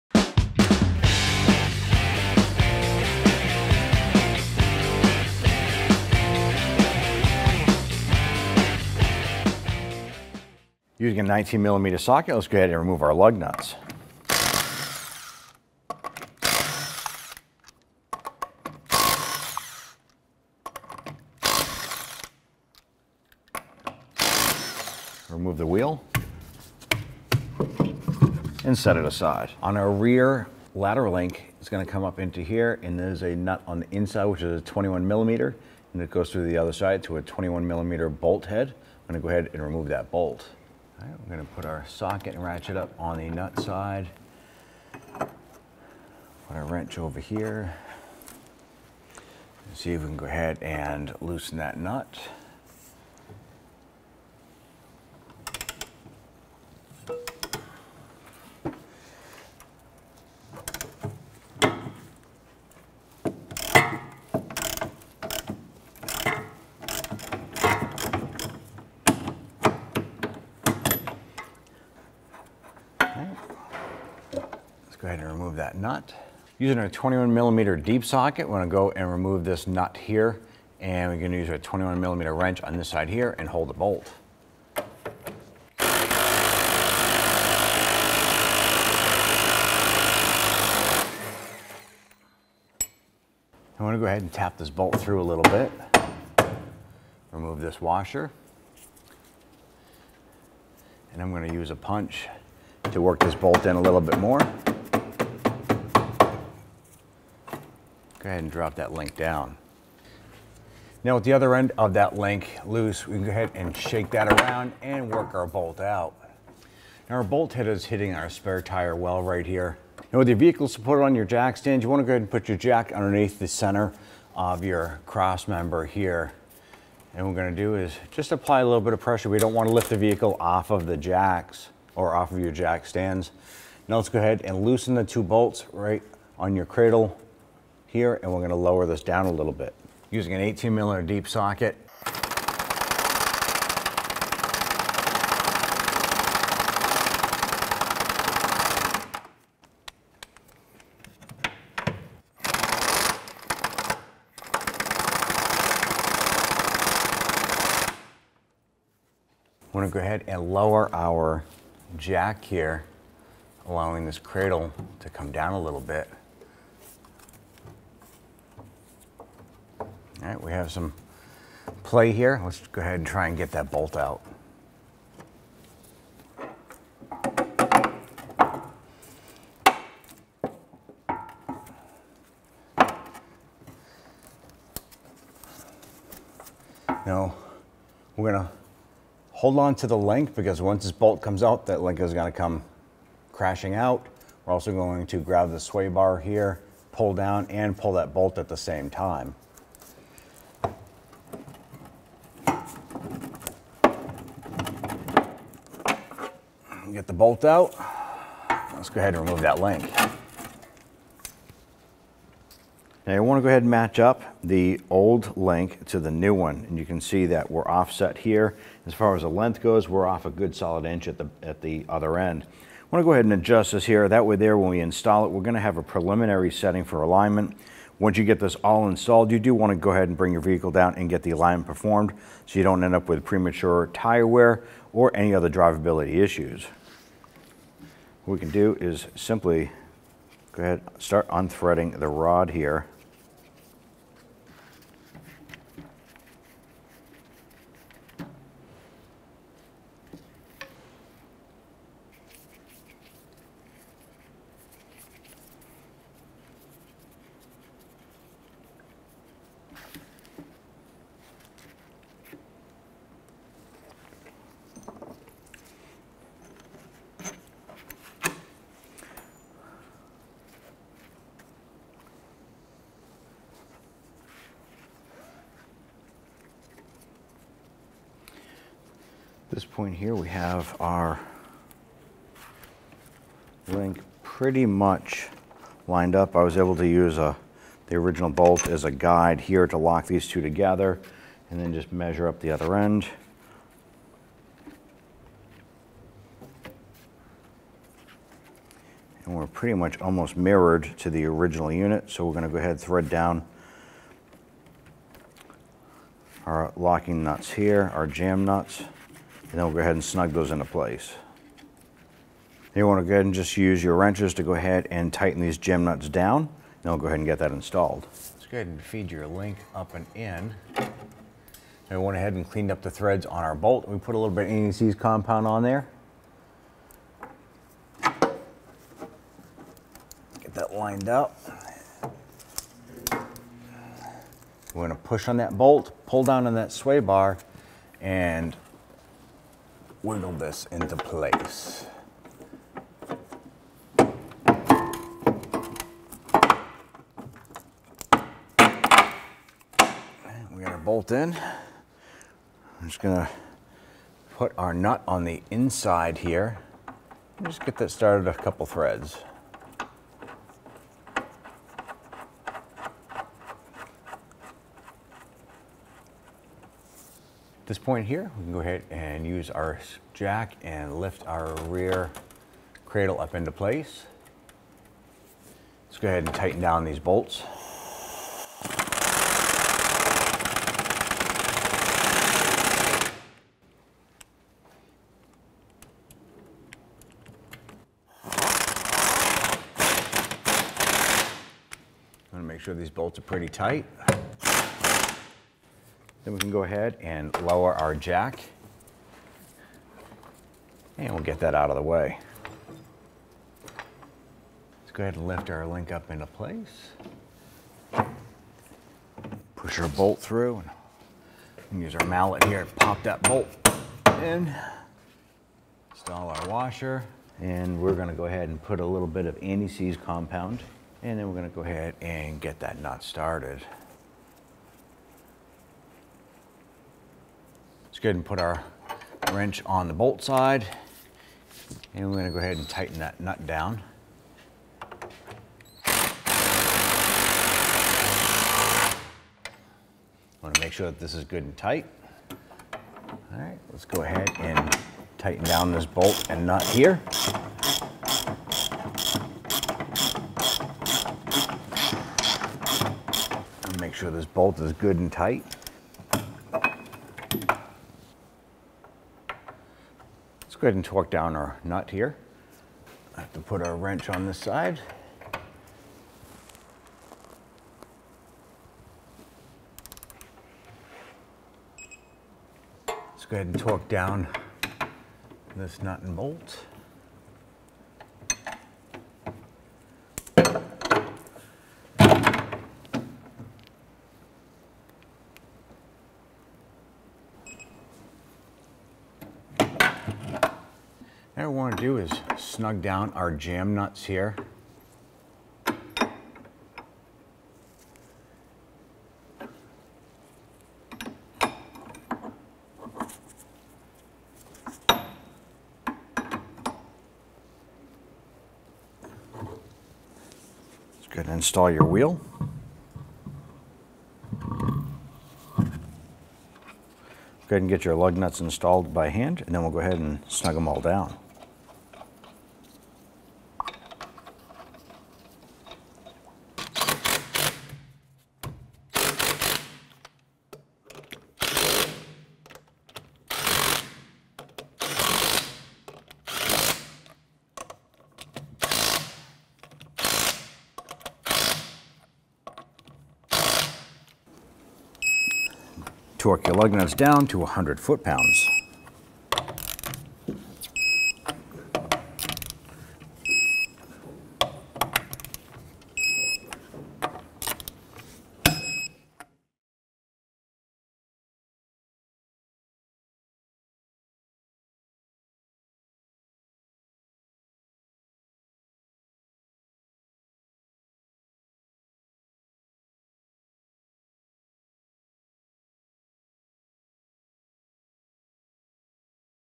How to Replace Rear Lateral Link 2006-2013 Chevrolet Impala
This video shows you how to install a rear lateral link on your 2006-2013 Chevrolet Impala.

This repair was done on a 2009 Chevrolet Impala LT 3.5L Sedan 4-Door FWD Automatic and the process should be similar on the following vehicles:
2006 Chevrolet Impala
2007 Chevrolet Impala
2008 Chevrolet Impala
2009 Chevrolet Impala
2010 Chevrolet Impala
2011 Chevrolet Impala
2012 Chevrolet Impala
2013 Chevrolet Impala

Tools you will need:
• 21mm wrench
• 18mm Socket
• 19mm Socket
• 21mm Socket
• Rubber Mallet